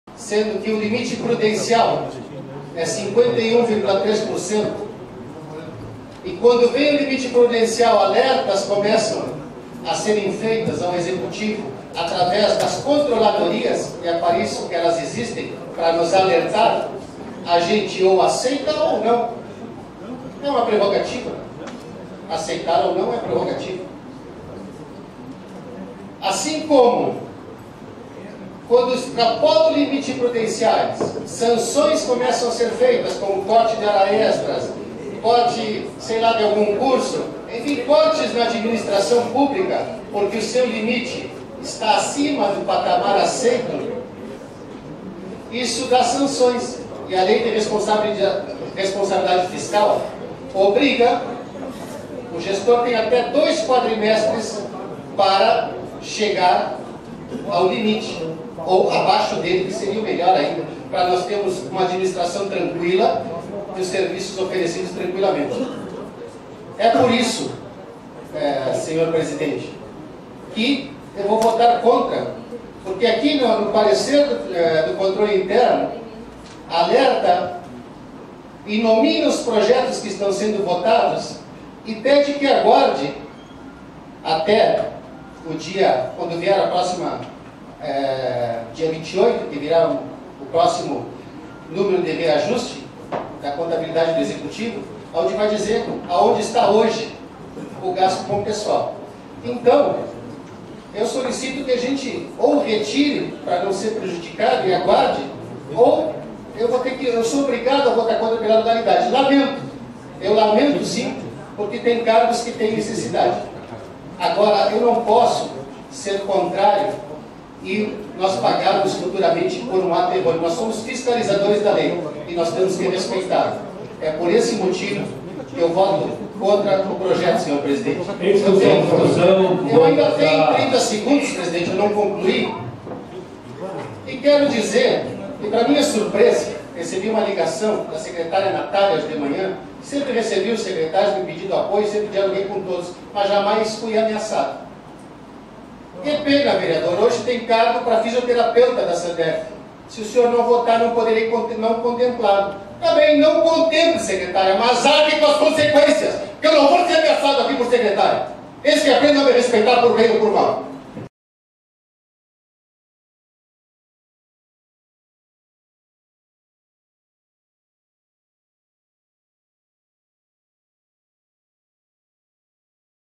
Nilo - Vereador
El vereador Nilo, en su discurso durante la sección de esta mañana denunció que recibio amenanzas de la Secretaria de Salud de la Prefeitura de Livramento. Visiblemente molesto por la situación fue contundente